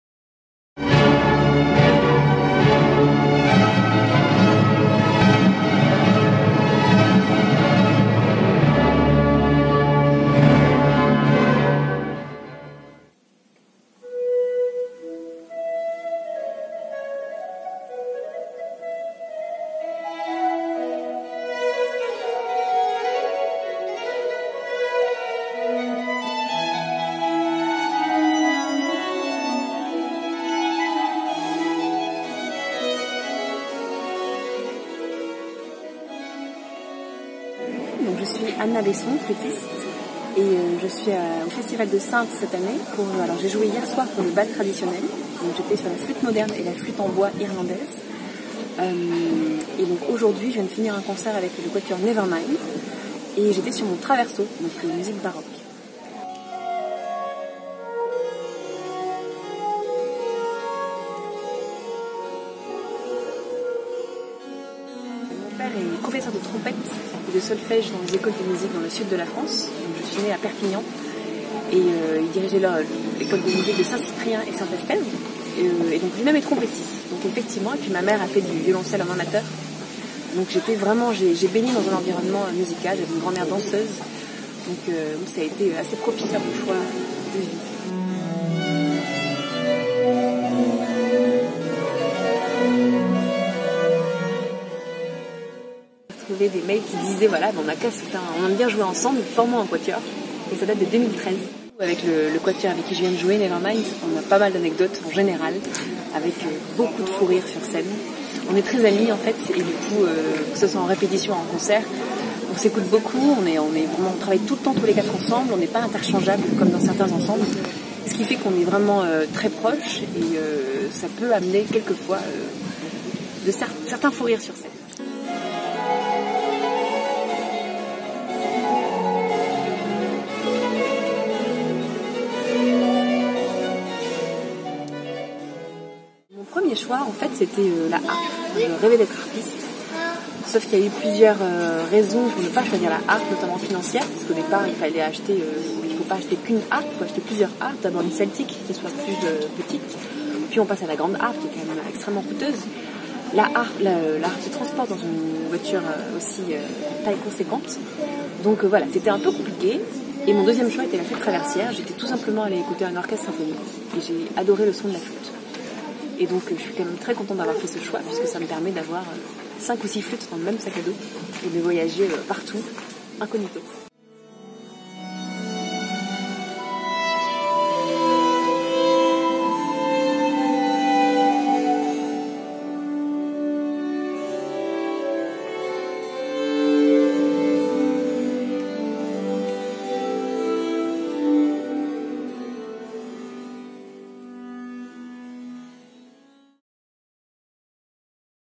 Anna Besson Flutiste enchantée
Vidéo réalisée dans le cadre de l'atelier Média de l'Abbaye aux dames - Festival de Saintes